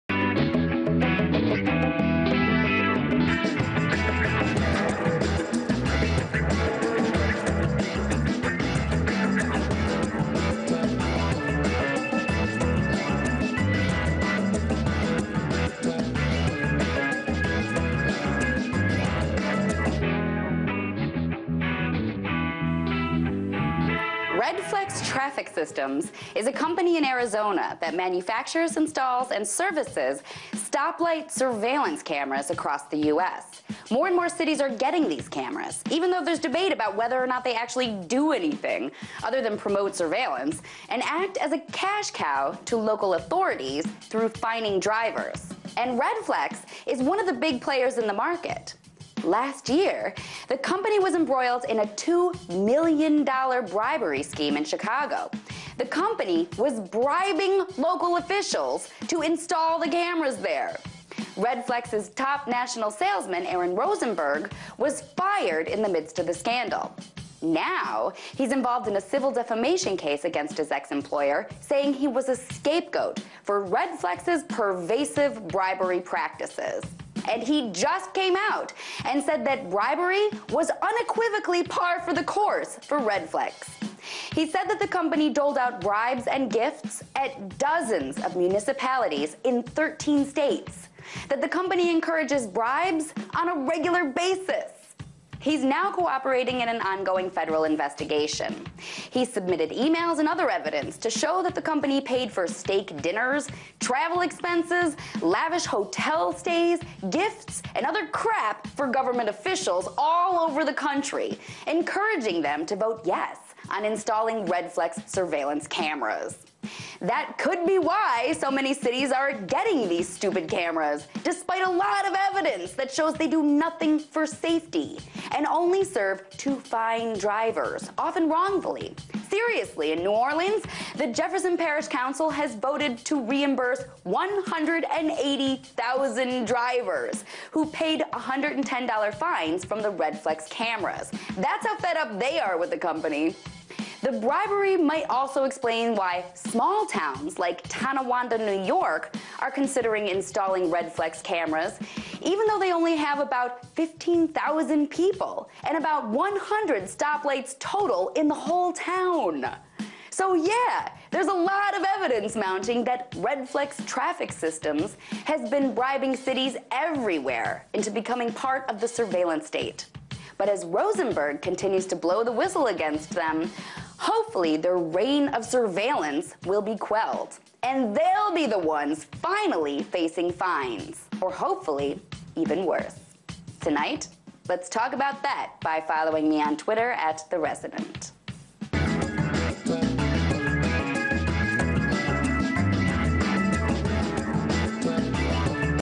Redflex bribing cities to install surveillance cameras
Redflex Traffic Systems was embroiled in a $2 million bribery scandal in Chicago last year, and one of their top execs, Aaron Rosenberg, was fired amidst the scandal. Now, he's working with the feds to expose how the company has been regularly bribing city officials across the nation to encourage them to install stop light camera systems. The Resident (aka Lori Harfenist) discusses. Follow The Resident at

Find RT America in your area: /
Or watch us online: /